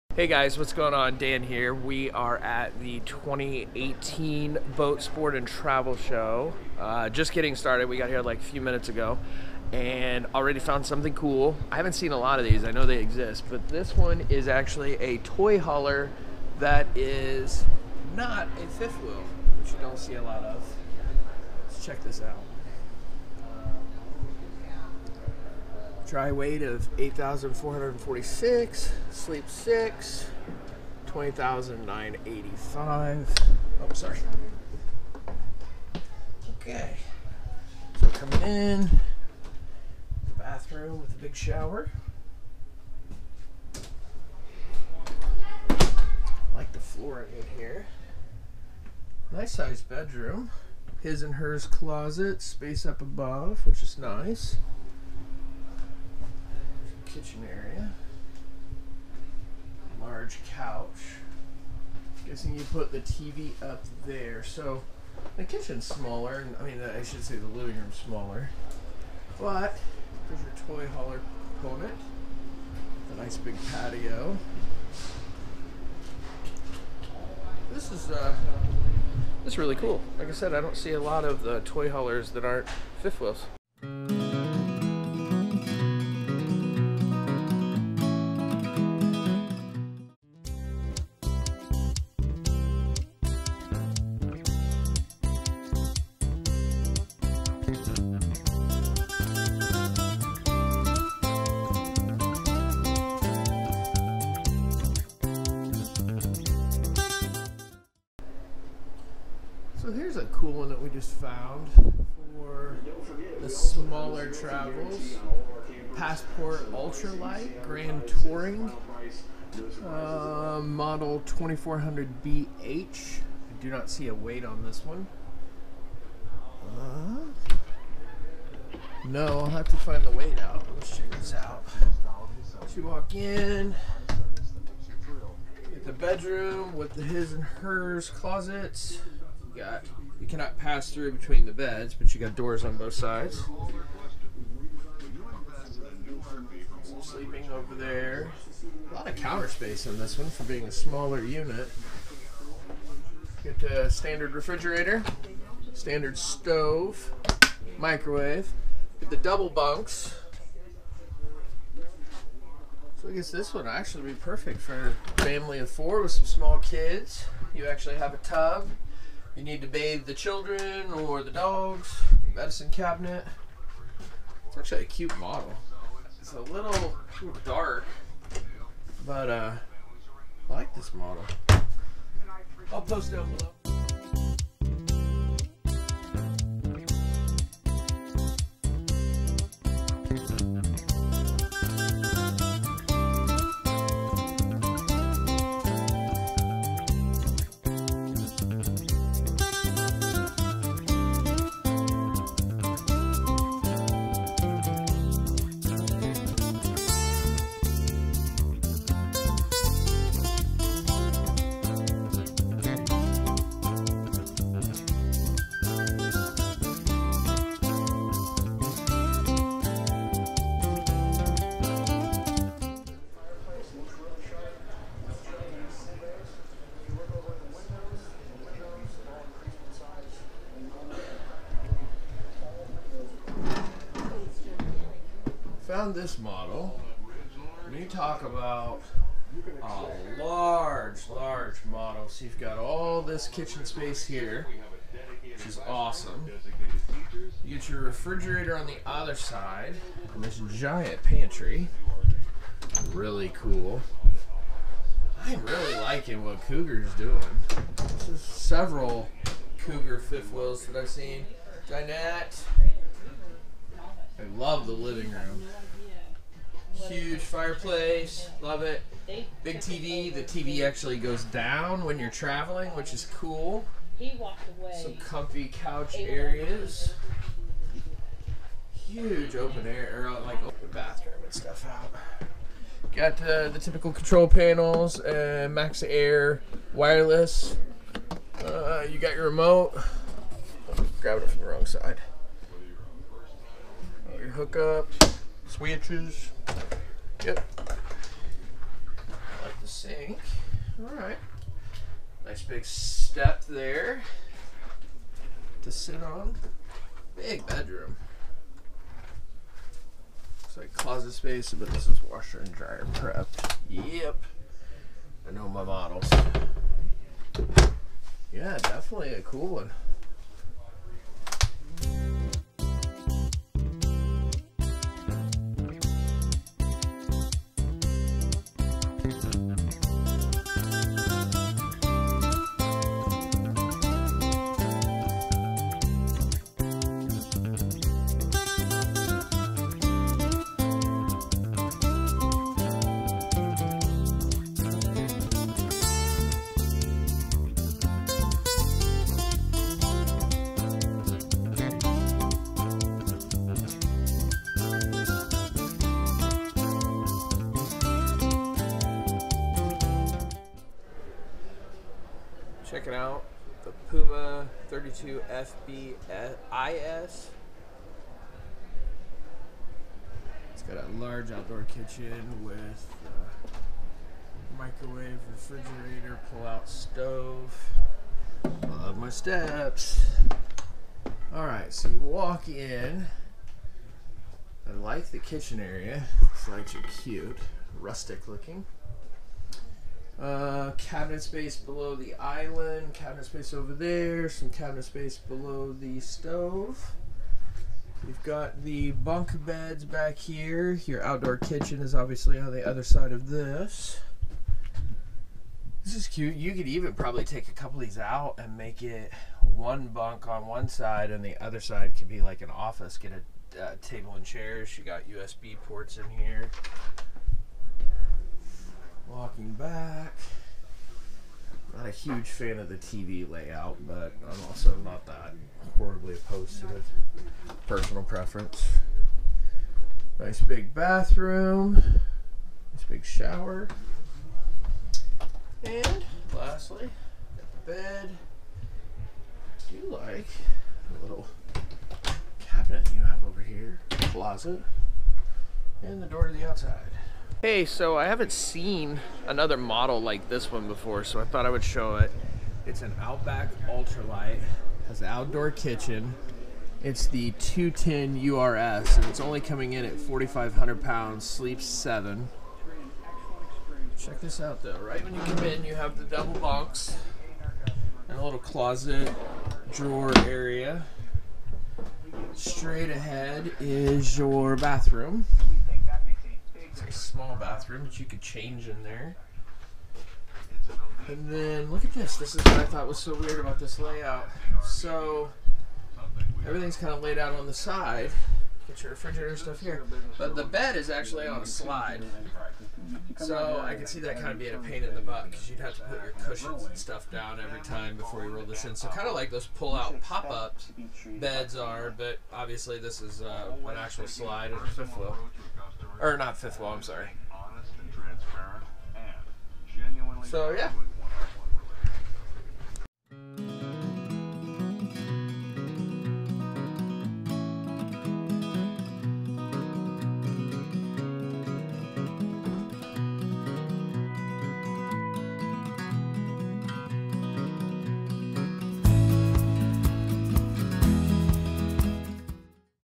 2018 Indianapolis Boat Sport & Travel Show - Travel Trailers, Fifth-Wheels, Boats, and more!
We visited the 2018 Indianapolis Boat Sport and Travel show last week. It's always a favorite time for me. Not only are there hundreds of campers to tour, but also boats, watercraft, ATV's, and TONS of fishing supplies!
In this newest video we tour several different travel trailers and fifth-wheels. We have: Pumas, Cougar, Montana, and more featured.